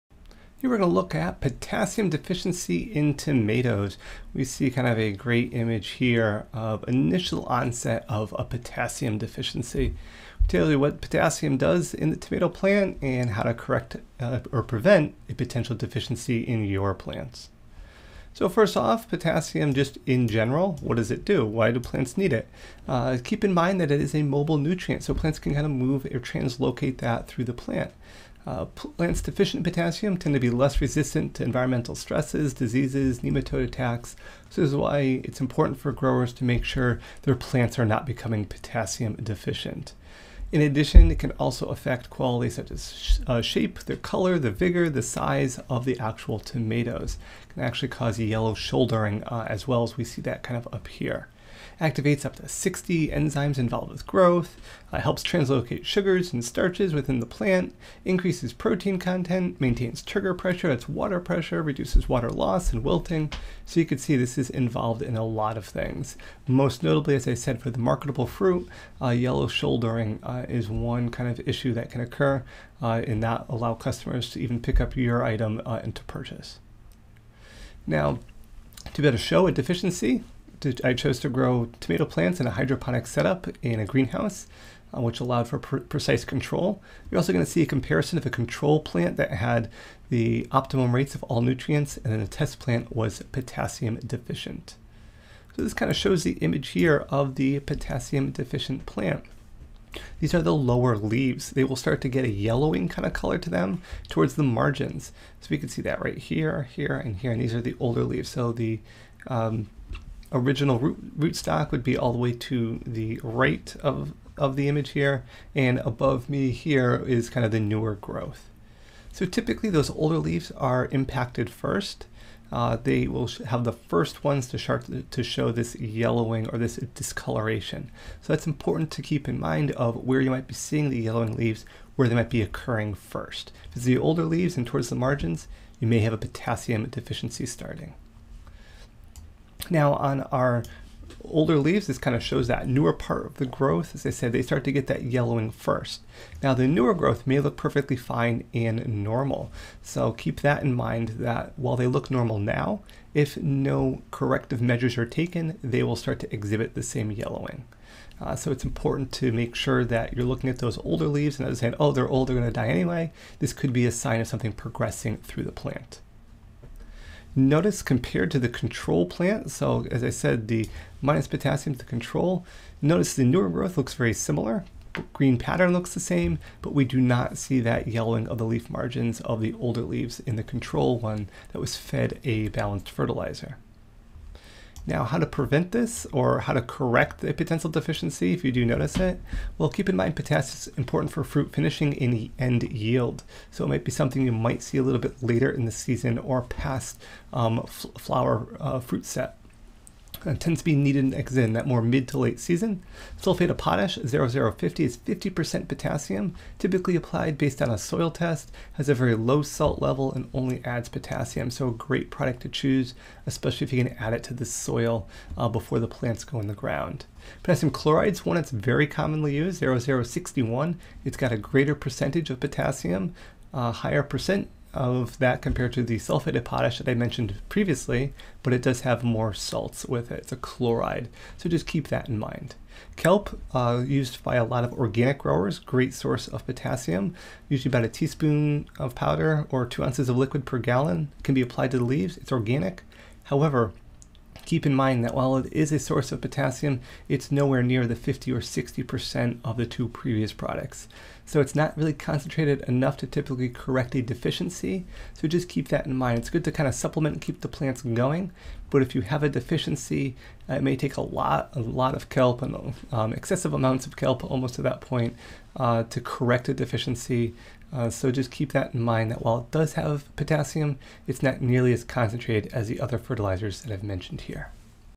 Potassium Deficiency in Tomatoes
Potassium Deficiency Look For...

-The lower (older) leaves will start to get yellowing leaves toward the margins.
-Typically, the leaves furthest from the main stem will show the yellowing first.

Correcting a Potassium Deficiency Options...
Sulfate of potash (0-0-50):
Rate: Based on soil test
Pros: Low salt level
Cons: Only adds potassium

Potassium chloride (0-0-61):
Pros: High K %
Cons: Increased salt level

Kelp:
Rate: 1 tsp of powder (or 2oz of liquid) per gallon when applied to leaves
Pros: Organic
Cons: Not concentrated enough to typically correct a deficiency

*Due to the description character limit the full work cited for "Potassium Deficiency in Tomatoes" can be viewed